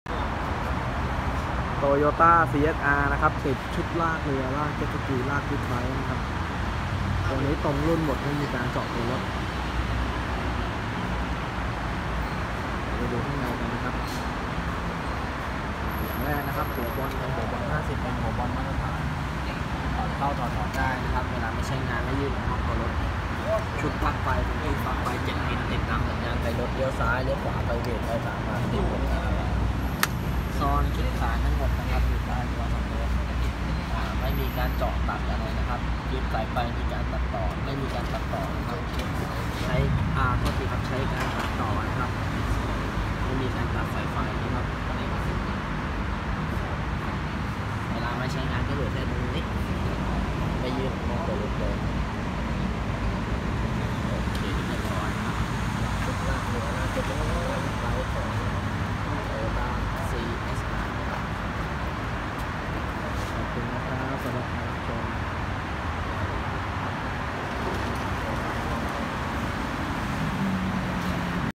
Toyota CHR ติดตั้ง ชุดลากเรือ CHR ตรงรุ่น ลากพ่วง ซีเอชอาร์ คานลากเรือ เทรลเลอร์ลากพ่วง
💥 ขอบคุณลูกค้า เข้าใช้บริการติดตั้ง ชุดลากเรือ CHR ชุดลากพ่วง ซีเอชอาร์ ไม่เจาะตัวรถ ซ่อนคาน ถอดเข้าออกได้
✏️ Inbox (กล่องข้อความ) : m.me/thuleclub